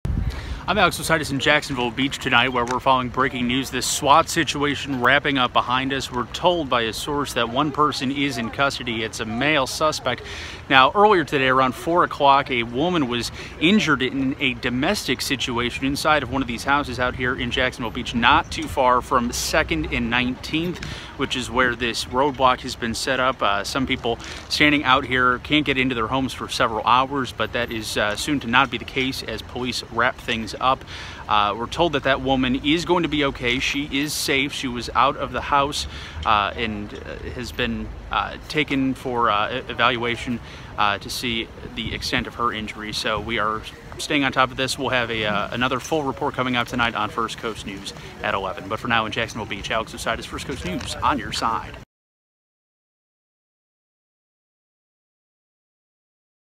One person in custody following SWAT call in Jacksonville Beach
One man is in custody after a domestic battery was reported in a Jacksonville Beach neighborhood.